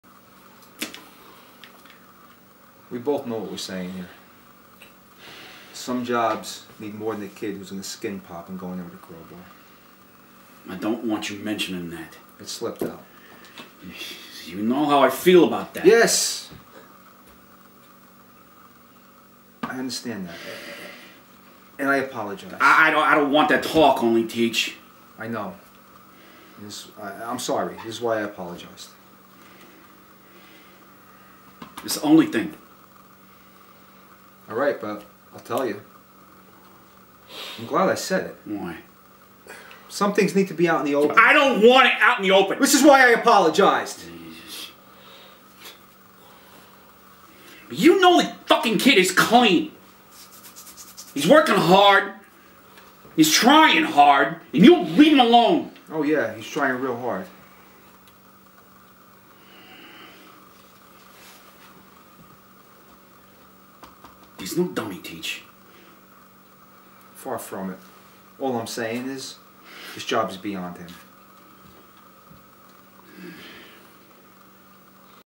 Vincent Curatola’s Acting Workshop BTS
Actor Vincent Curatola host's acting classes for both the seasoned and inexperienced!